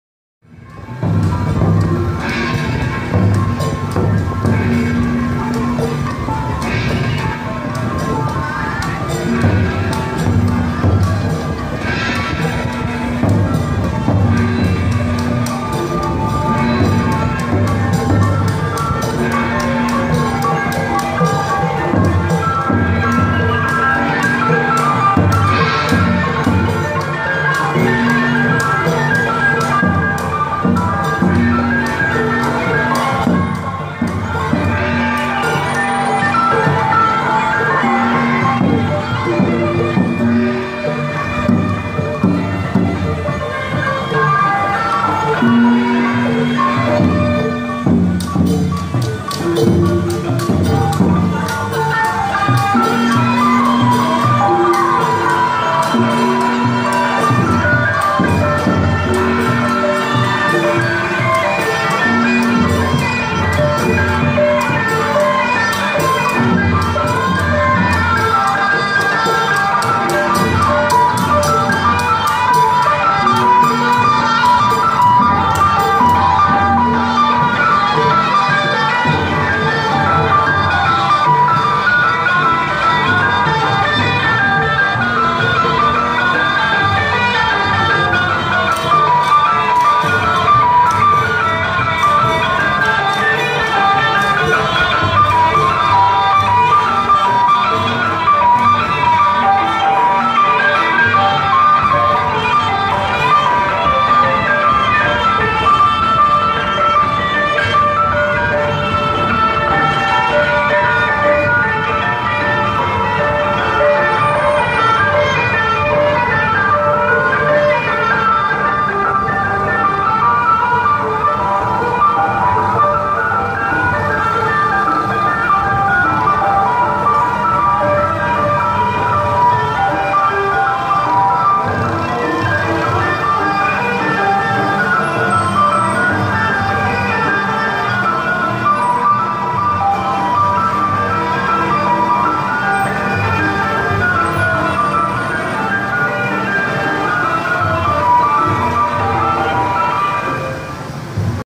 A parade comes by in Phnom Penh Cambodia(1)
I was sitting on the balcony enjoying the afternoon breeze when a parade came through.